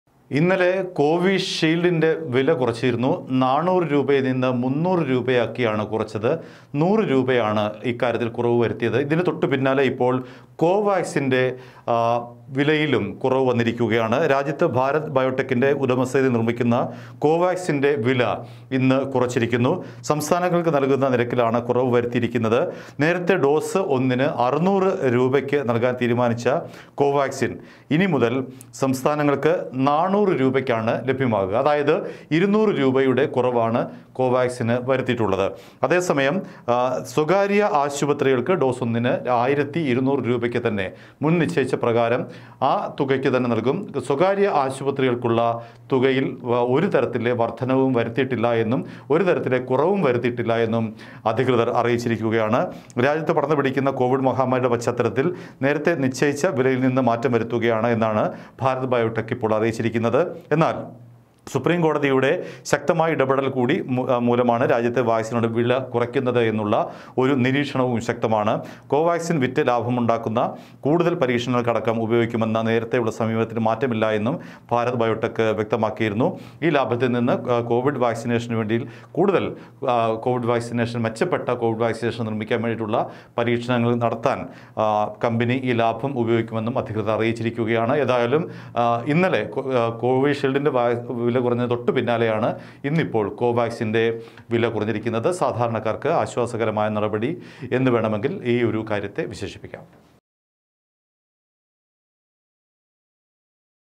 കോവാക്‌സിന്‍ 200 രൂപ കുറച്ച് സംസ്ഥാനങ്ങള്‍ക്ക് നല്‍കും l Covaxin rate
600 രൂപ വിലവരുന്ന കോവാക്‌സിന്റെ വില 200 രൂപ കുറച്ചതായി അധികൃതര്‍. സര്‍ക്കാരിന് നല്‍കുന്ന ഡോസിന്റെ വിലയാണ് കുറച്ചത്. സ്വകാര്യആശുപത്രികള്‍ക്ക് 12,00 രൂപക്ക് തന്നെ നല്‍കും